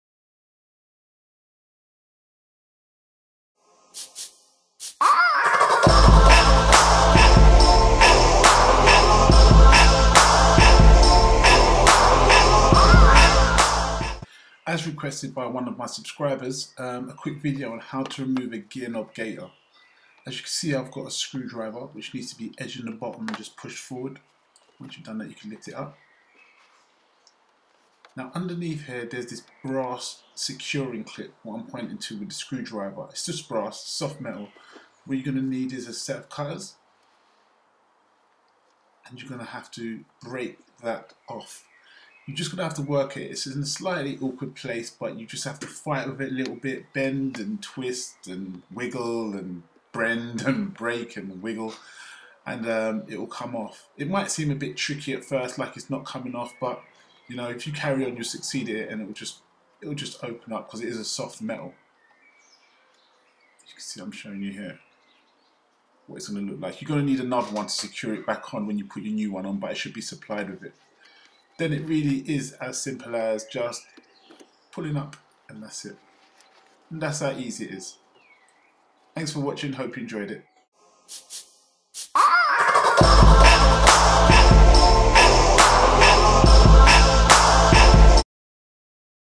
VW Gear Knob Gaitor Boot Removal as Requested by Subscriber
How to Remove A VW Golf Mk4 Gear knob Gaitor Boot, Simple easy steps,